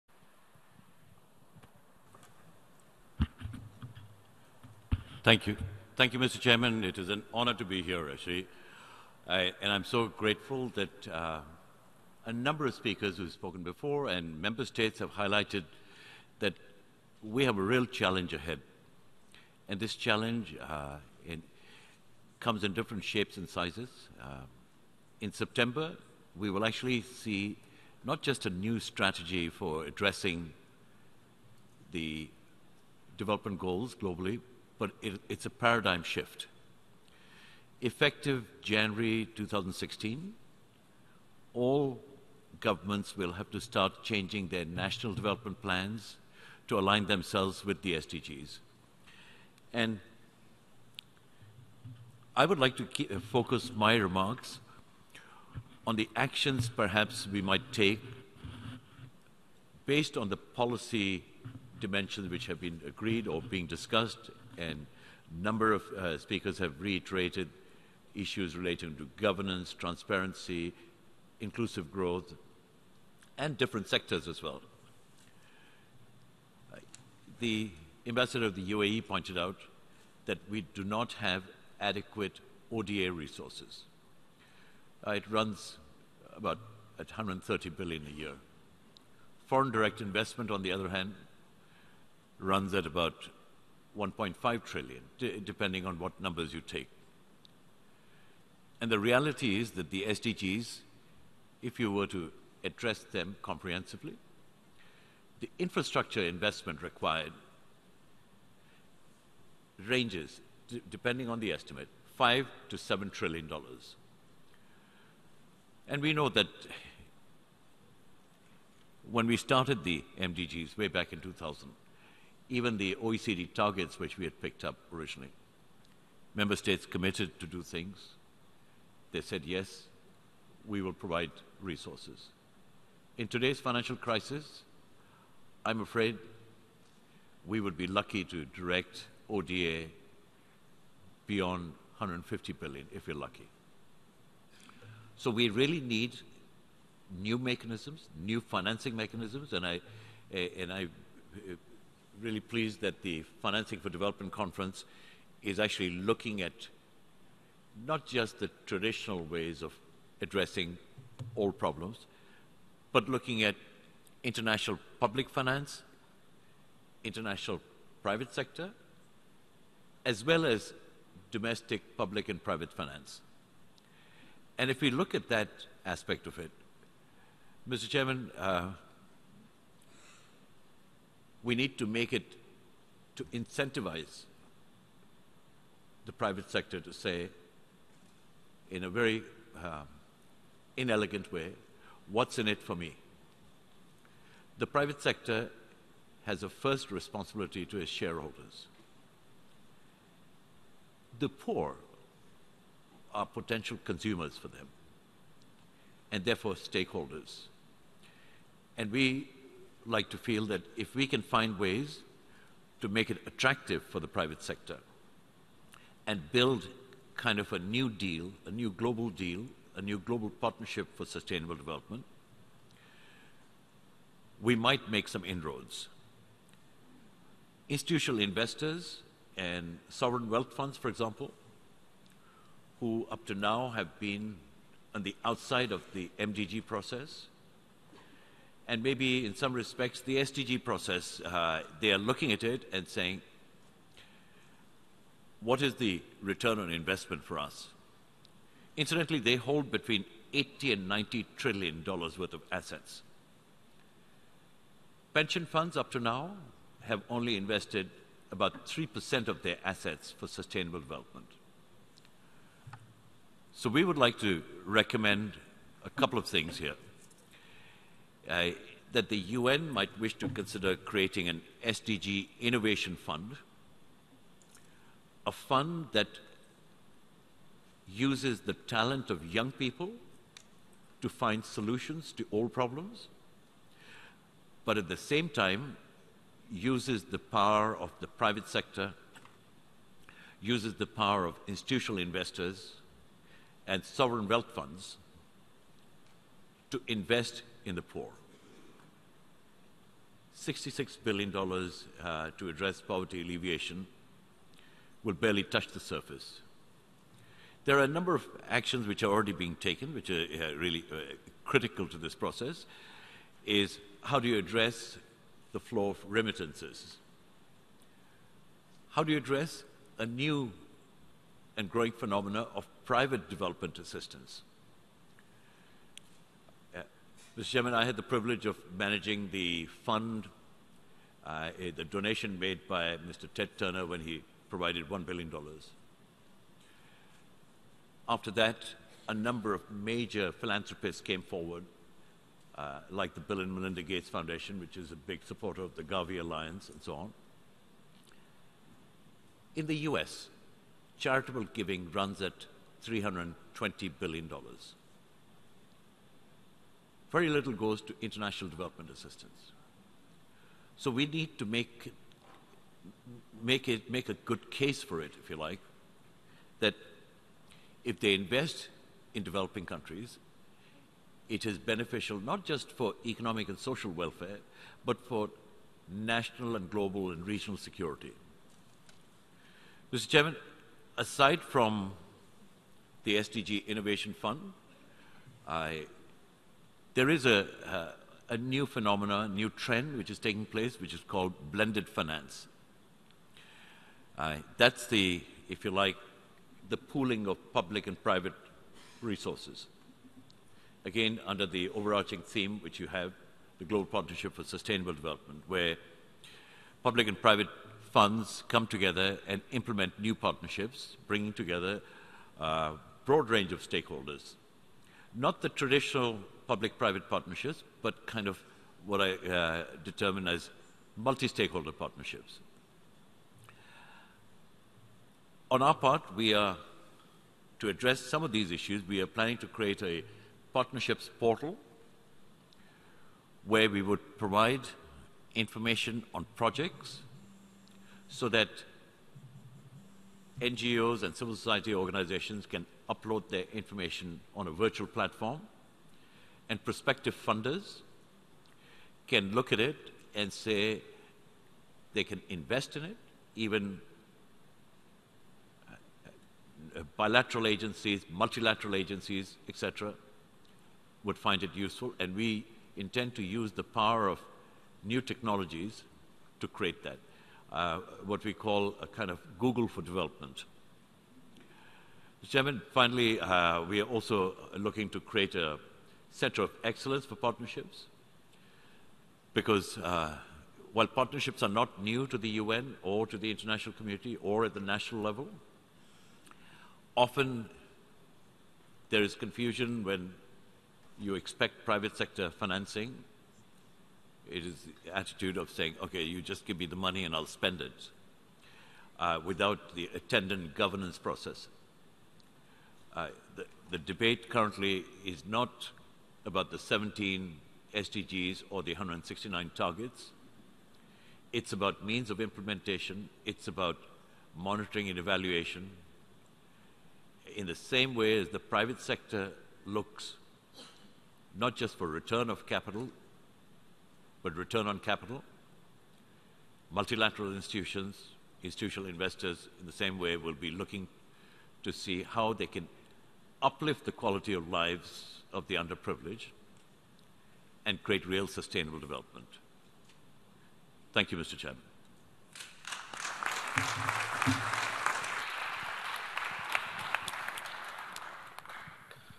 Mr. Amir Dossal - Global Partnerships Forum - Post-2015 Interactive Hearings
Speech by Mr. Amir Dossal, Global Partnerships Forum, during Roundtable Discussion 3 of the Hearings of the General Assembly with NGOs, civil society organizations, major groups and the private sector in preparation for the United Nations summit for the adoption of the post-2015 development agenda on 27 May 2015. Mr. Amir Dossal is British living in the USA.

For more information about the event, including concept note, programme and statements, please